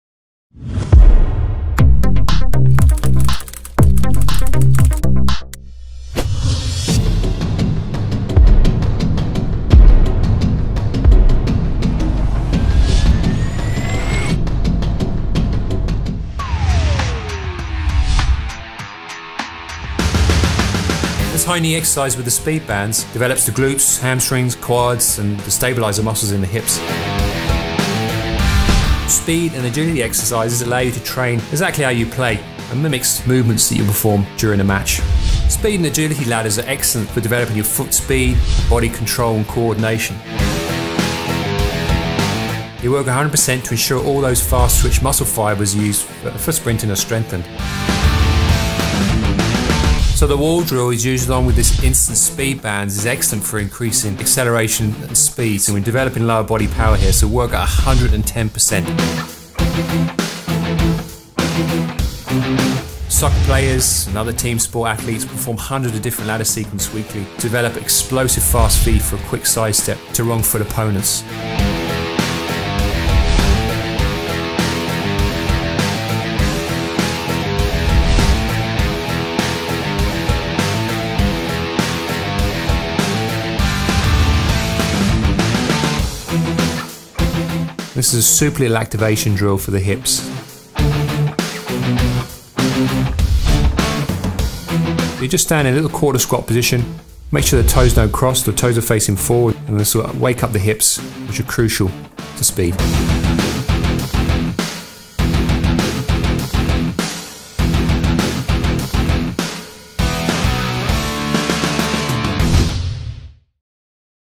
Instant Speed Training | Speed Bands | Increase Speed
The benefits of resistance training for increasing speed have been well documented in the past but now there is a product to achieve results fast.

Speed Bands ensure the leg muscles work against the resistance of the bands. This activates the fast-twitch muscle fibres in the hamstrings, quads, glutes and core muscles to help you increase your speed, explosive power and balance. The High Knee Wall Drill is a great exercise for training these muscle fibers in your legs. Visit our Video Blog to view all our training videos.

Using the Speed Bands while doing ladder work and other speed and agility drills will develop strength in your legs for that quick side-step to wrong foot your opponents and accelerate on faster. When the bands are removed, you will notice the feeling of lightness and quickness in your legs and feet and be primed to accelerate quicker up and down the field.

All drills and exercises should be firstly performed using the bands and then repeated immediately after without the bands when your legs feel light and strong.

The main advantage of the Speed Bands is that they can be worn while training for all multi-directional movements. This ensures all muscles that your sport requires are targeted and strengthened.